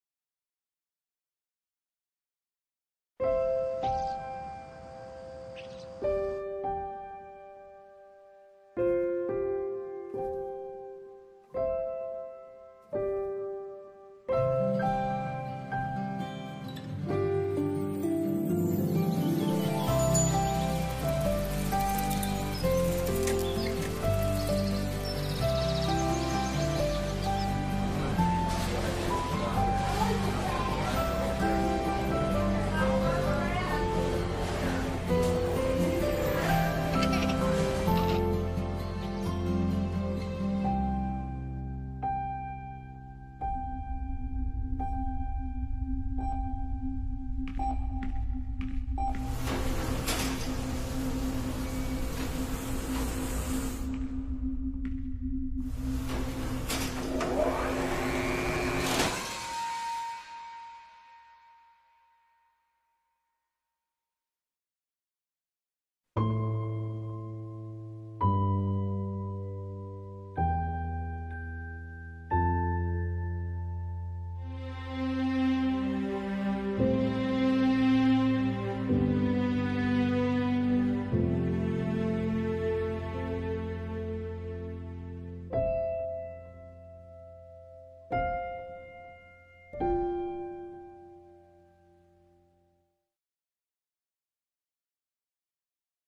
Best Safety Motivation video 2021
Work Safety Video by WSA, Casting Andi Lin and my dearest gal Clovelle Foo.

Source from Workplace Safety and Health Council:
This TV commercial is Inspired by a true event where a worker had his arm amputated resulting from an accident while working with machines. Touching on the emotional bond between a father and daughter, this commercial aims to drive home the message that accidents at the workplace will hurt not only yourself but also the people around you. The commercial concludes with the campaign message “Prevent all injuries. Go home safe and healthy".